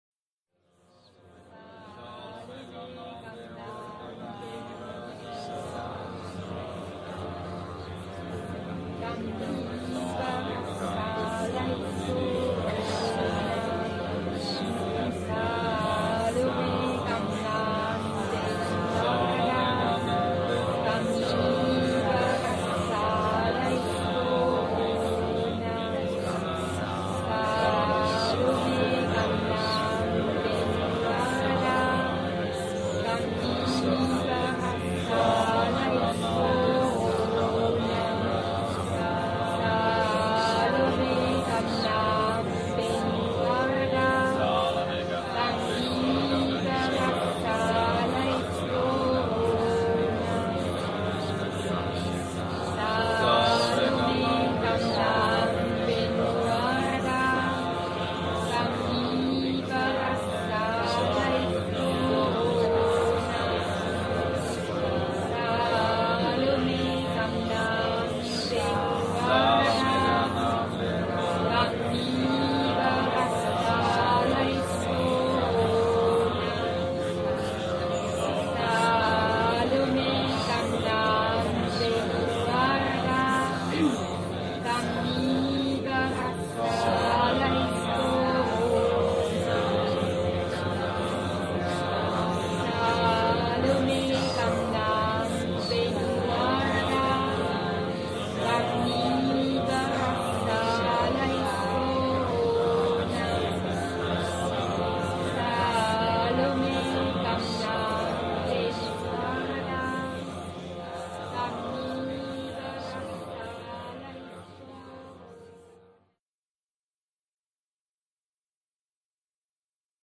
Friedensmeditation (Hörprobe)
Aufnahme einer Friedensmeditation im Semjase-Silver-Star-Center